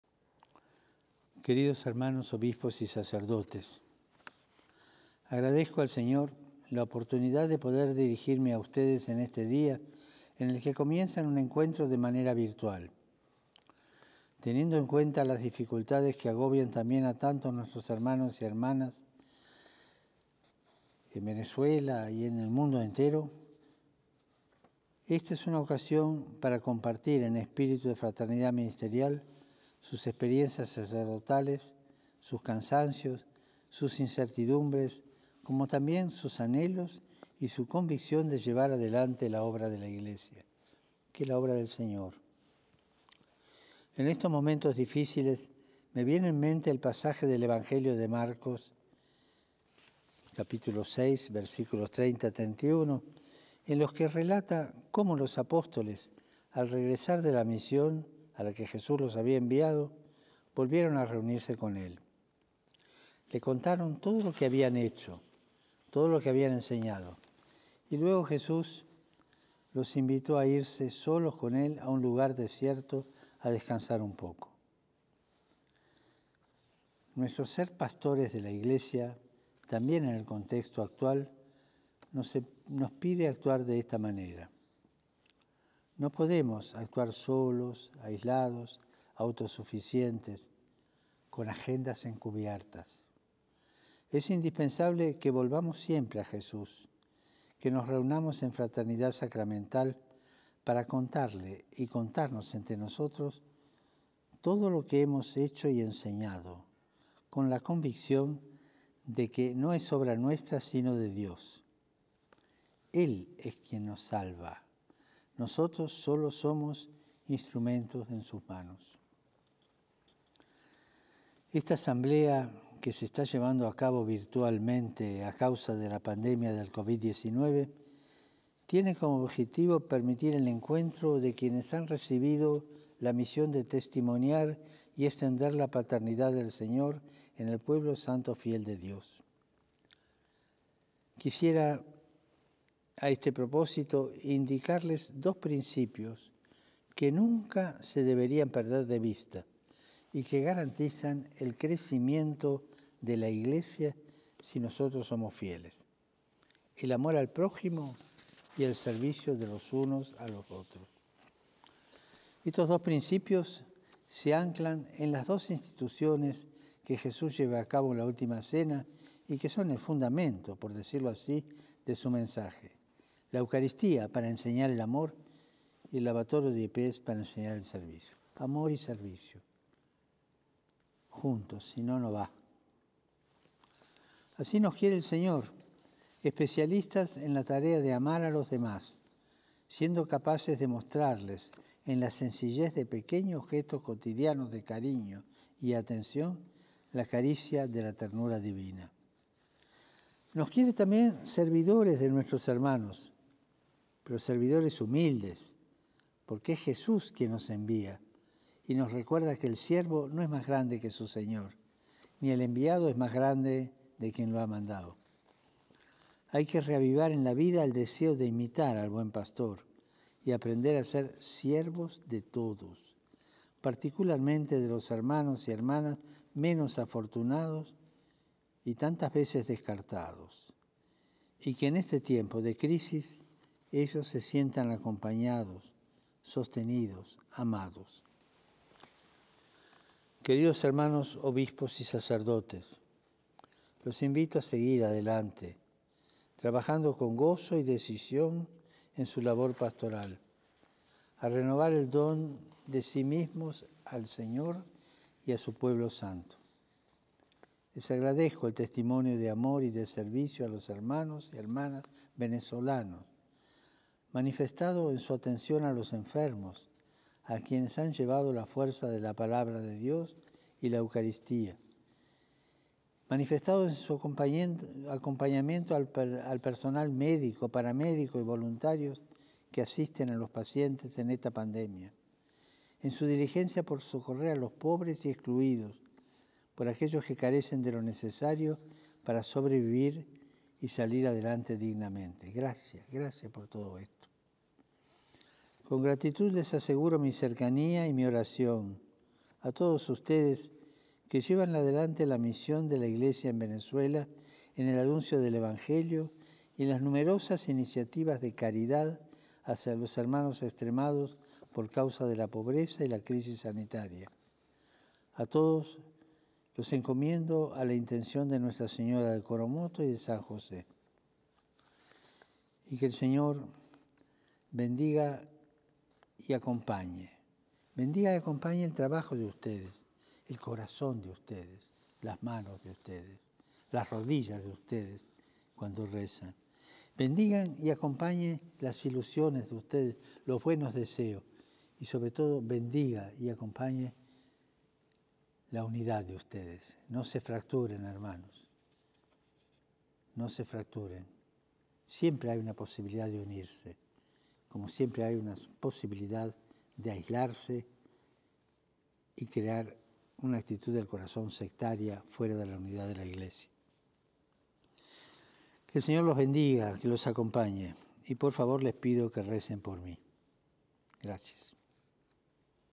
Videomensaje del Papa Francisco a participantes del encuentro de Obispos y sacerdotes de Venezuela
Les dijo: "Así nos quiere el Señor: especialistas en la tarea de amar a los demás, siendo capaces de mostrarles en la sencillez de pequeños gestos cotidianos de cariño y atención, la caricia de la ternura divina".